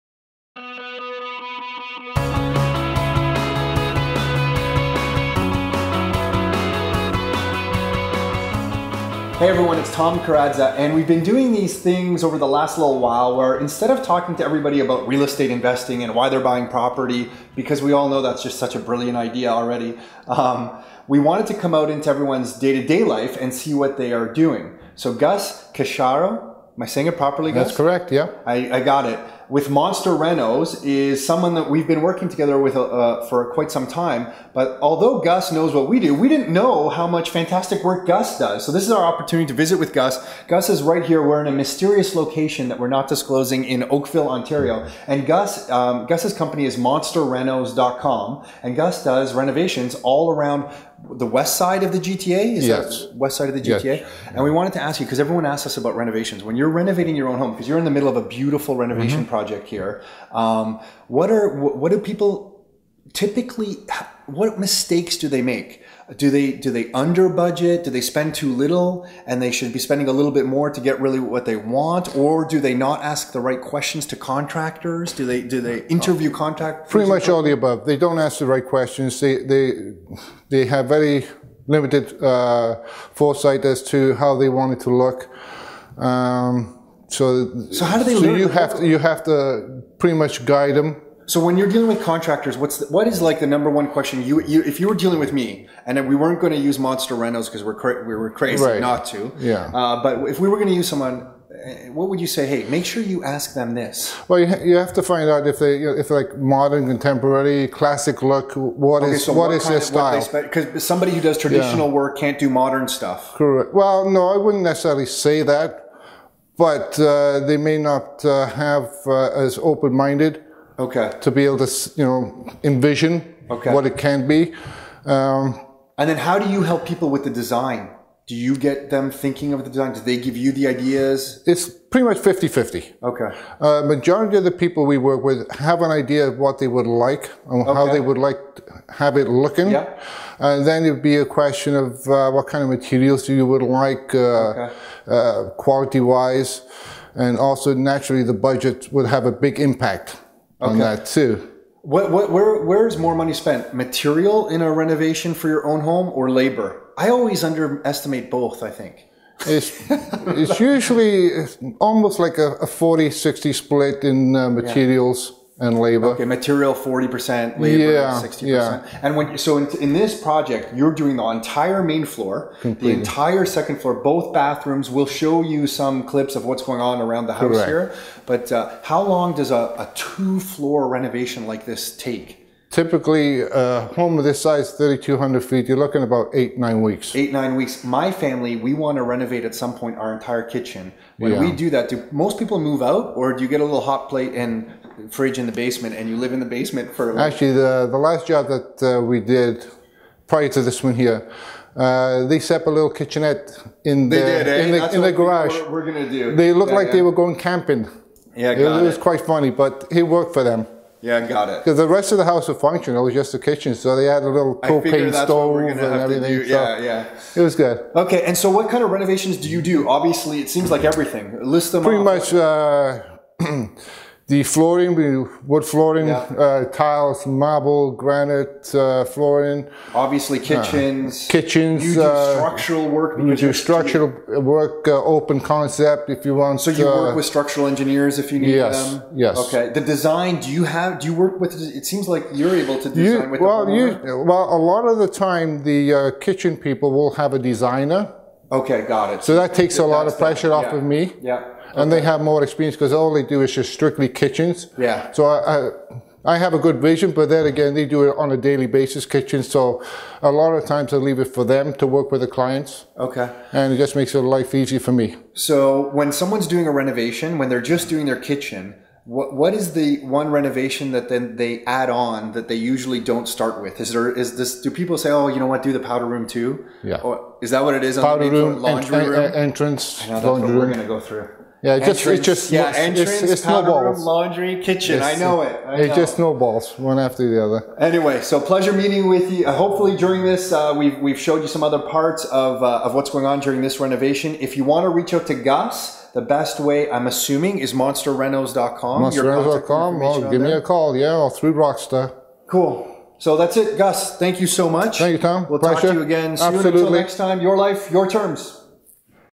MonsterRenos.com with Gus Casciaro
Tom visited Rock Star member Gus Casciaro as he was onsite working one of his current renovation projects to discuss Gus' business. If you're interested in any home renovation work, check out for more information.